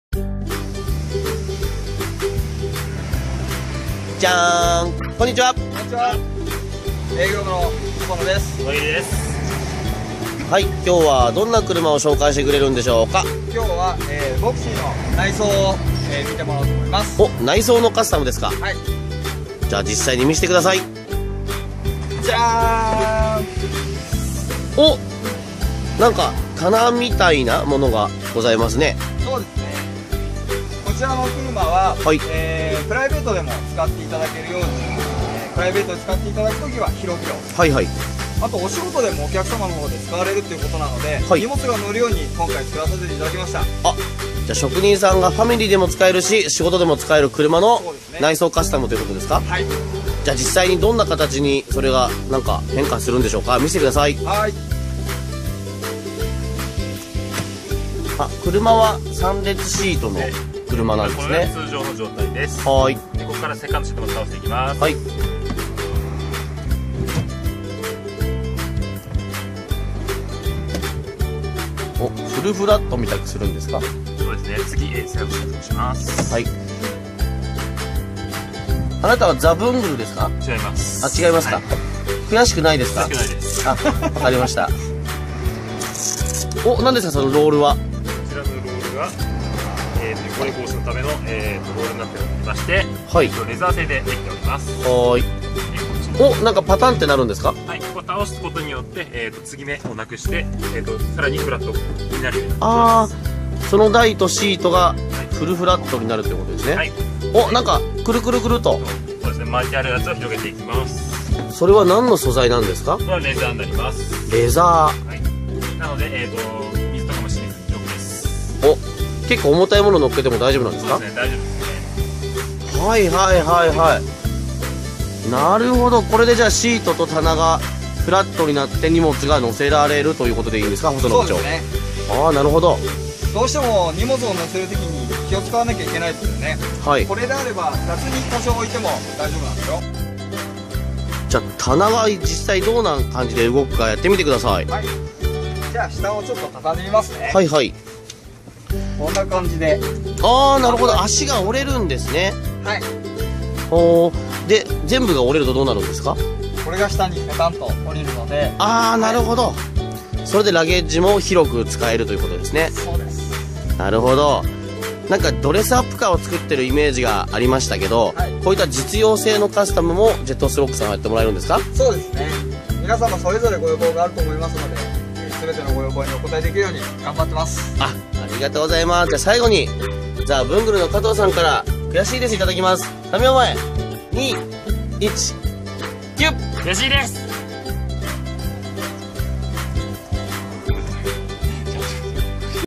職人さんのご要望をカタチに！！
プライベートでもお仕事でもどちらでも使える取り外しが簡単な棚&シートを専用設計で製作しました！